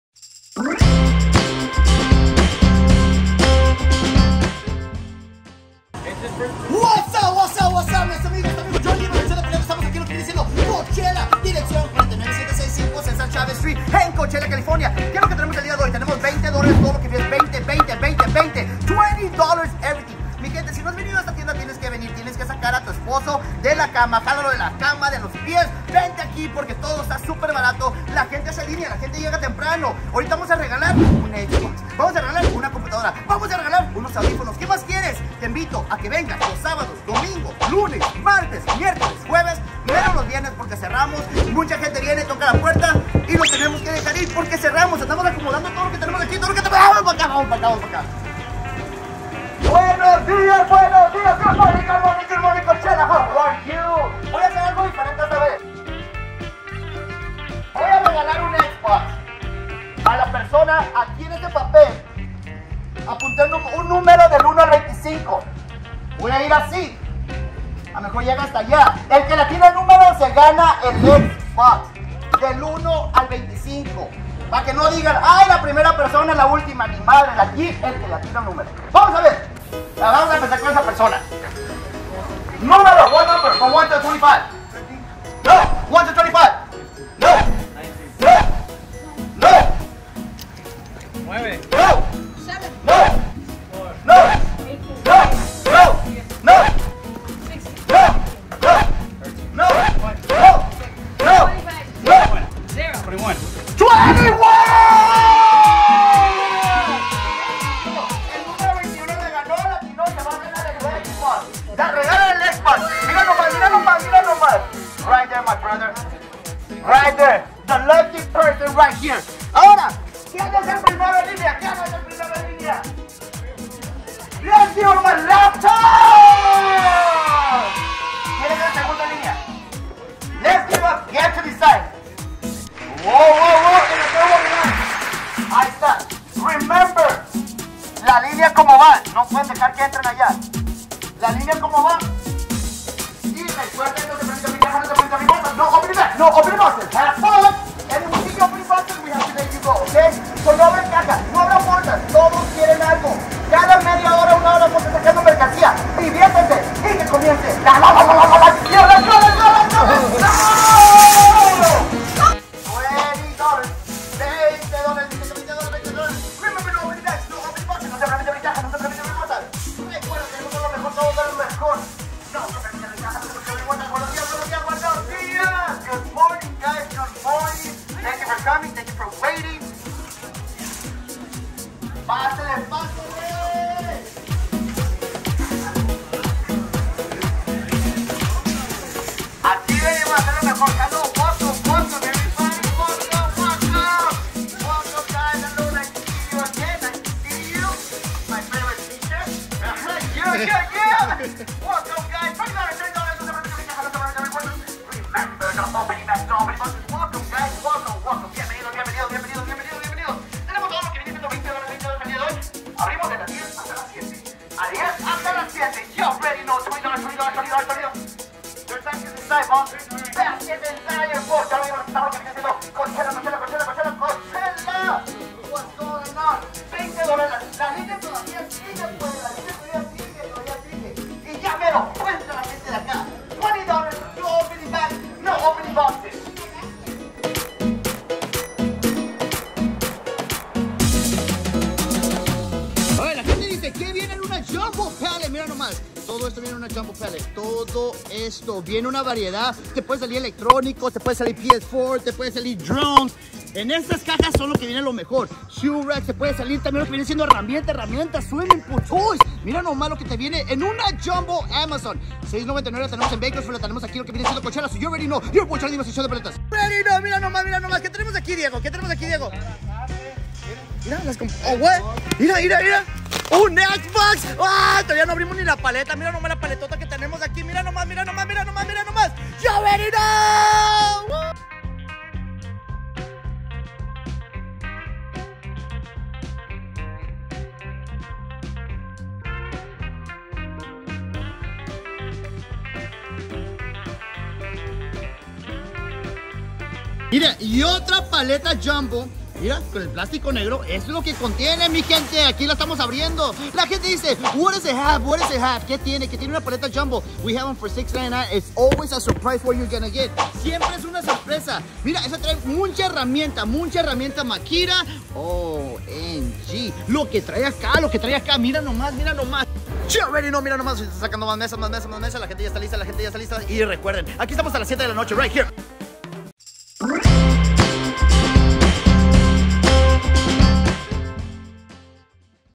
¡NO SE LO ESPERABAN! SORTEO de un XBOX a los clientes en fila
Te esperamos con una gran variedad de productos a precios de LOCURAAAAAAAAAAAAAAAAAAAAAAAAAAAA!!!

Address
4580 State rd
Bakersfield ca